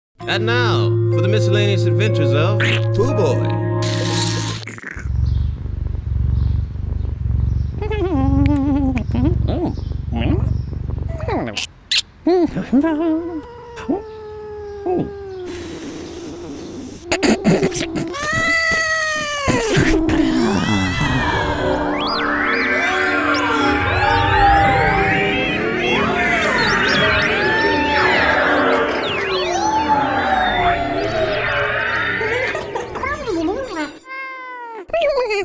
Poo Boy: Doobie Cat
Our hero, Poo Boy, learns the truth about kitty cats and cat nip.